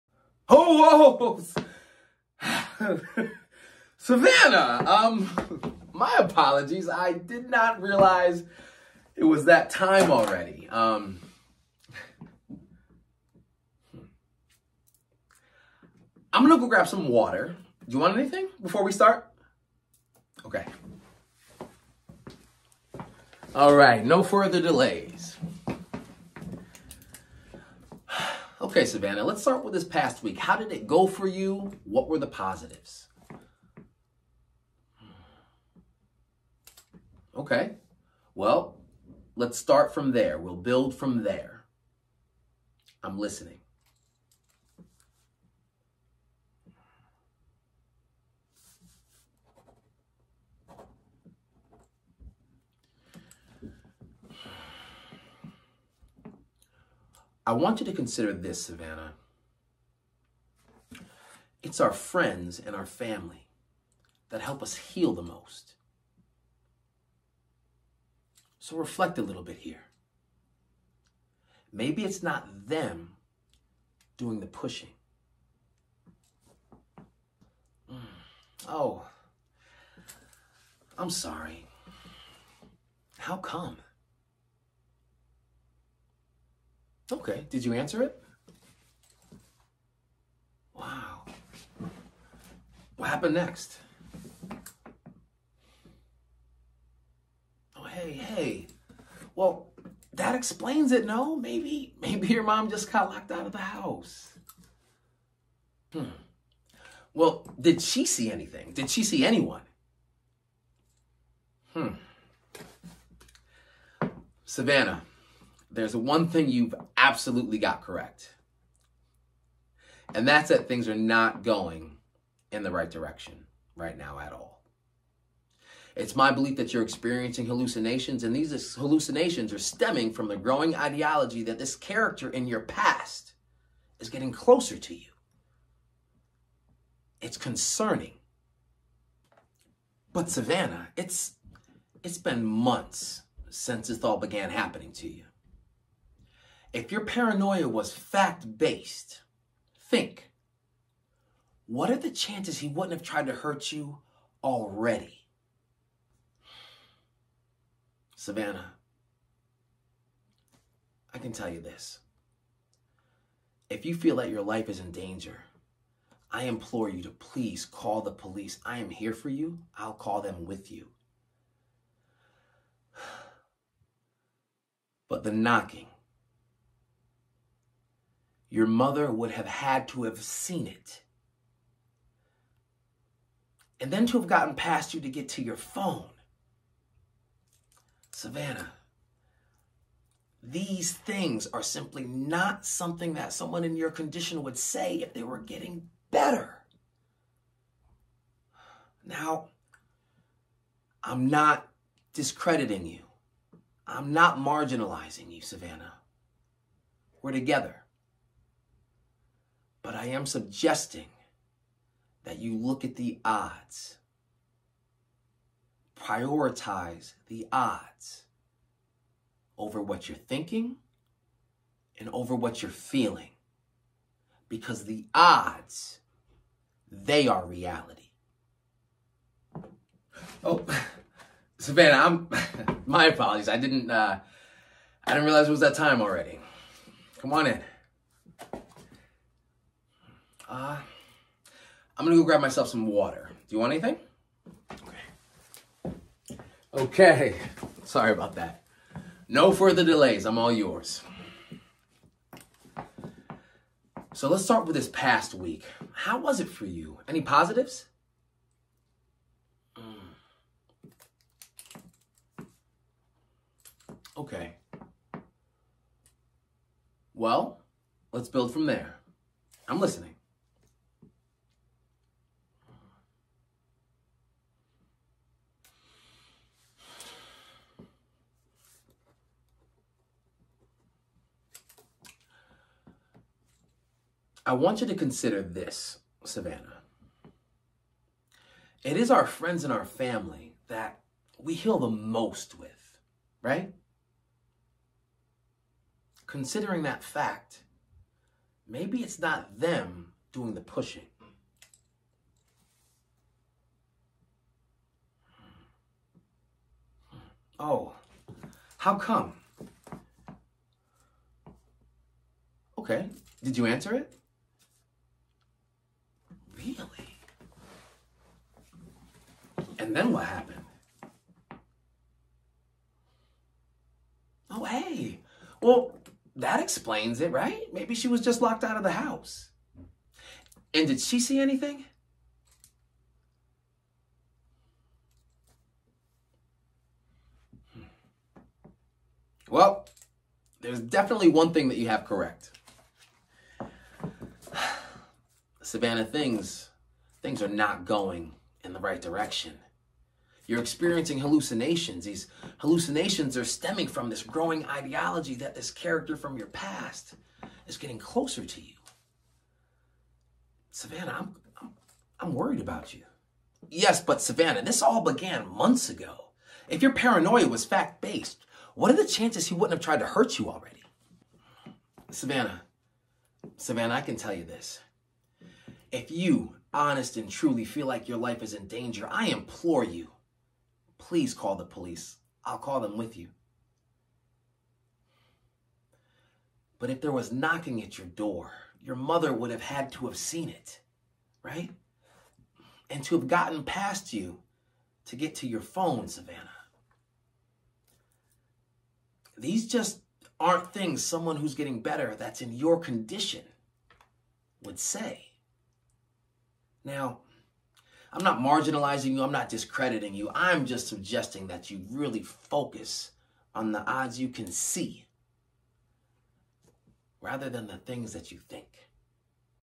Derek Fox : Role - Dr. Mark (Beacon of Hope & Understanding/Twisted)
Script: "No One's Listening"
Feature Film

 PLOT:
A young woman, struggling to cope after a traumatic experience, finds her reality unraveling as she seeks therapy, leading her to question whether her growing paranoia is a descent into madness or a response to a real but unseen threat.

ROLE DESCRIPTION:
DR MARK - A therapist who initially appears to be a beacon of hope and understanding for Savannah, yet harbors dark secrets. His personal tragedy and subsequent twisted motivations slowly unravel, revealing his true connection to John and his role in the continuation of Savannah's torment. His character serves as a critique of the trust placed in authority figures and the complexities of ethical conduct in therapy.
Dr. Mark is staring at the black picture frame in his hand. Savannah walks to the door frame and doesn't say anything as he stares at the photo. After waiting a moment, Savannah knocks. This startles Dr. Mark.

Dr Mark
Oh Savannah. My apologies, I didn't realize it was that time already.

         - Dr Mark puts the picture down

Savannah
Yeah, I guess a week goes by pretty fast when you have nothing to look forward to.

          -Dr Mark gets up

Dr. Mark
I'm going to grab myself some water; you want anything?

Savannah
No, I'm fine thanks.

         -Dr Mark exits the room. Savannah begins looking around the room. She notices a calendar       on the wall, and a few notes on the desk. She then fixates her gaze onto the black picture frame. Suddenly reappears Dr Mark

Dr Mark
Now, with no further delays
(Sits in his chair)
How has the past week been for you? Any positives?

Savannah
Positives No. Negatives? Oh, yeah.

Dr Mark
Well, lets build from there... I listening

Savannah
I mean nothings changed. My mom still tries to hard... and my friends push too hard.

          - Savannah looks down

Savannah (cont'd)
I ended up blowing up on everyone

Dr Mark
Well, considering our family and friends are the ones who help us heal the most, maybe its you doing the pushing?

Savannah
They're not being very helpful. In fact nothing I've done to ...fix this, to fix me has been helpful, things have only gotten worse.

          -Dr Mark looks intrigued

Dr Mark
Oh? How come?

          - Savannah grows eerie

Savannah
Like a few days ago. mom leaves the house and as I am getting ready to head out there was someone knocking on the door.

Dr Mark
Did you answer it?

Savannah
Well, that's the thing. When I asked who it was, no one responded... they just... kept... banging

          - Dr Mark adjusts himself in the chair.

Dr Mark
And then what happened?

          -Savannah's eyes begin to water.

Savannah
I stated to walk towards the door... and by the time I opened it, my mom had walked in.

Dr Mark
Well, that explains it then. Maybe she was locked out the house.

Savannah
No, I asked her if she lest anything and she said no

Dr Mark
Hm. did she see anyone outside?

Savannah
That's why I think things are becoming worse. Because I know it was him. But my mom didn't see anything.

Dr Mark
Well, you've got one thing right.

          -Savannah looks slightly hopeful that Dr Mark shares her sentiments

Dr Mark
Things are not moving in the right direction. I think that you're experiencing hallucinations that may be stemming from the growing ideology that this character from your past is getting closer to you

Savannah
But, it's not the some 'ideology' I know its true. The police never did find his body.

Dr Mark
But Savannah, its been months since all of this happened to you. If your paranoia was fact based, what are the odds that he wouldn't have tried to hurt you already?

          - Savannah sits quietly...thinking

Savannah
I don't know. But what I do know is that I didn't make up the knocking at the door...or..or! The fact that when I got upstairs my phone wasn't where it was.

          -Dr Mark looks at her and doesn't say anything

Dr Mark
Savannah, I can tell you this, if you feel your life is endangered I implore you to reach out to the police. But if there was knocking at your door, your mom would have had to have seen it. And then to have gotten past you to get to your phone Those doesn't sound like things someone in your condition who's getting better would say. I'm not discrediting you, I am suggesting that you thing about the odds.